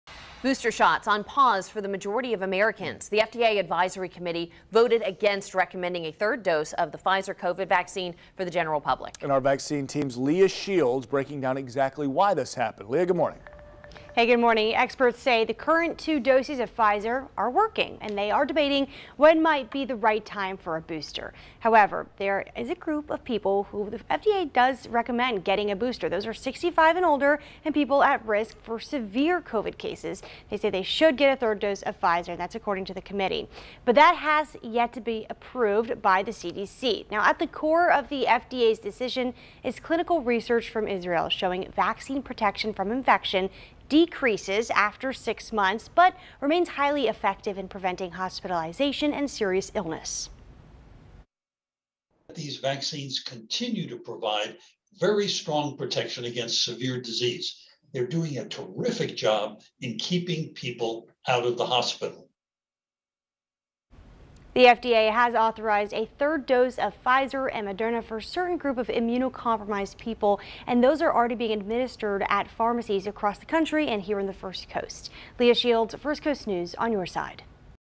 Booster shots are on hold for the majority of Americans
The FDA advisory committee voted against recommending a third dose of the Pfizer COVID-19 vaccine for the general public.